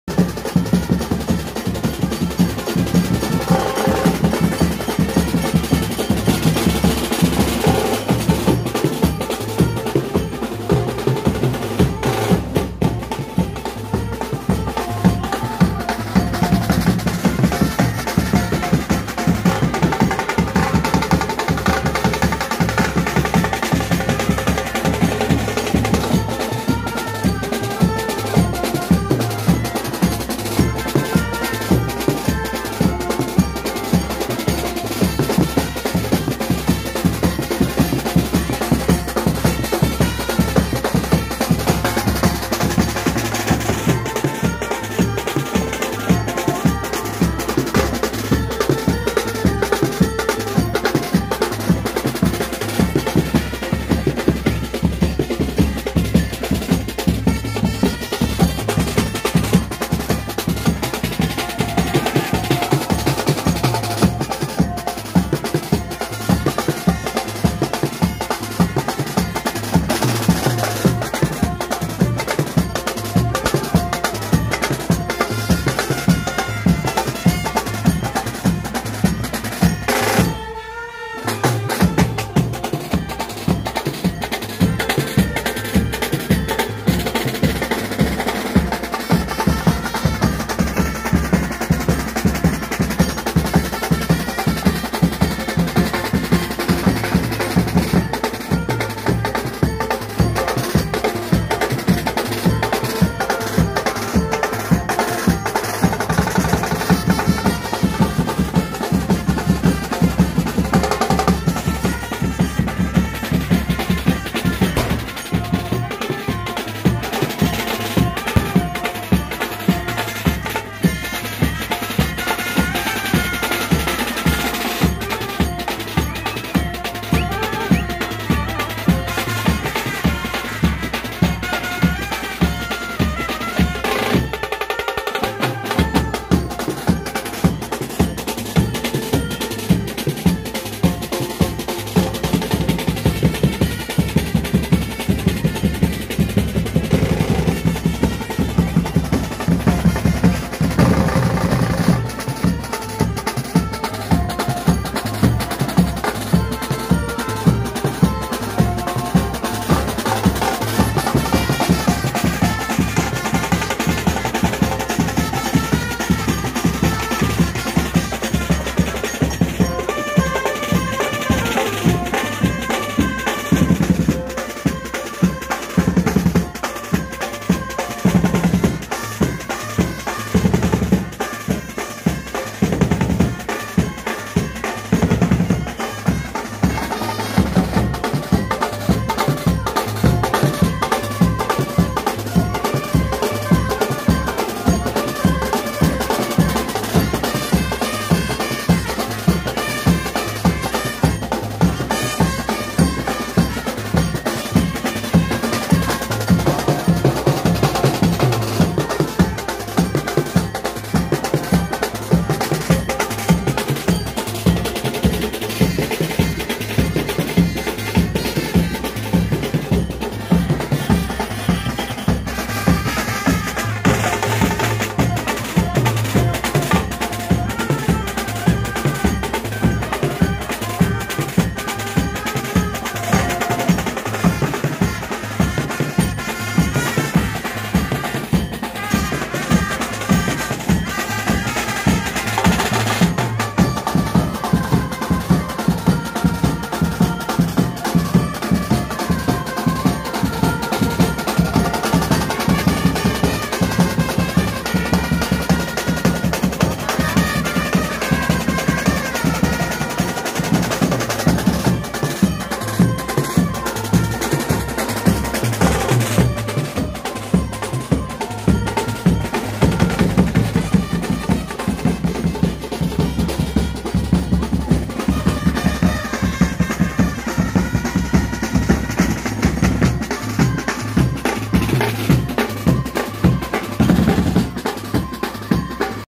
Lagn Varat ||AAI EKVIRA BRASS BAND KHONI BHIWANDI
जय एकविरा, जय आगरी कोळी
Missed You Tasha Player Sanket
Played :- AAI EKVIRA BEATS AND BAND KHO NI BHIWANDI
Place:-
2nd prize at bhandup competition